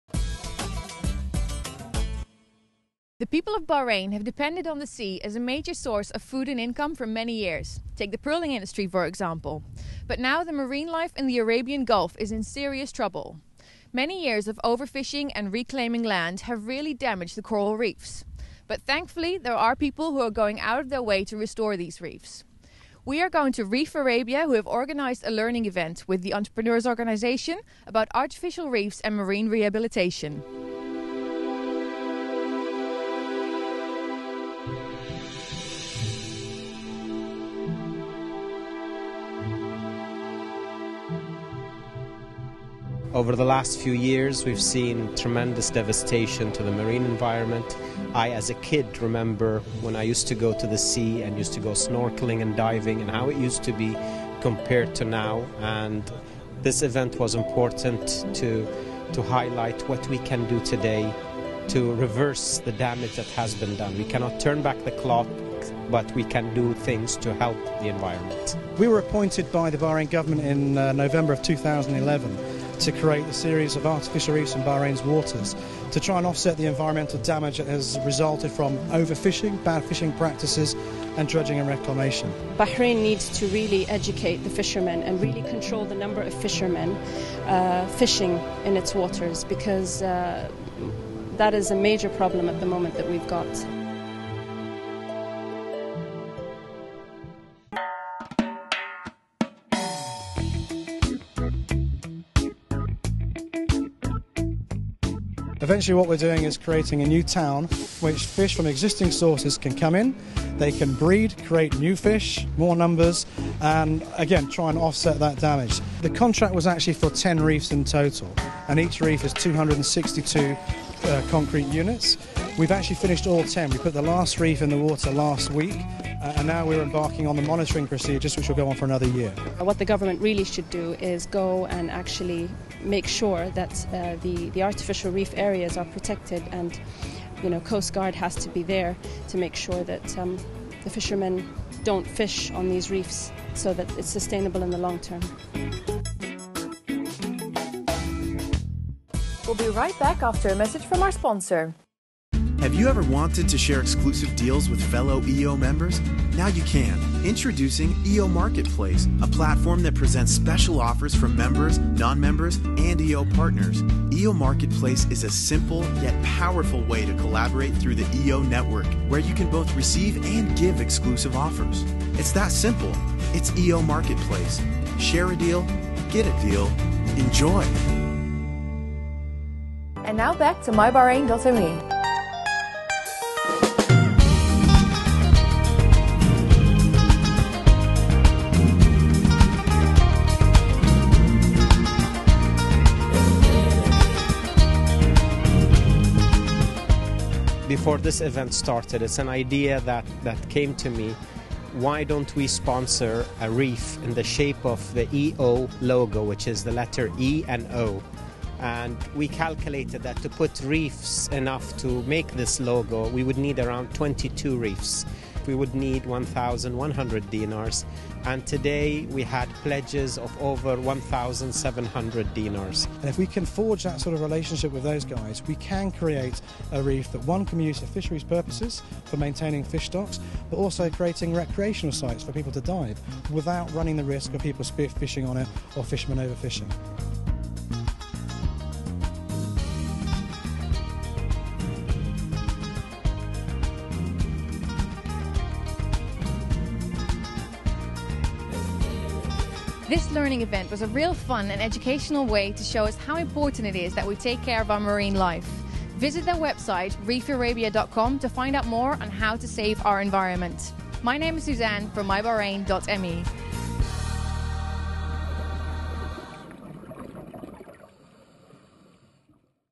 Reef Arabia: Artificial Reefs
Produced by Gulf Broadcast. for more. The marine life in the Arabian Gulf is in trouble. Years of over fishing and reclaiming land have seriously damaged the coral reefs in Bahraini waters. But thankfully there are people who are going out of their way to restore these reefs. Suzanne went to a learning event organized by Reef Arabia and the Entrepreneurs Organization about Artificial reefs and Marine rehabilitation.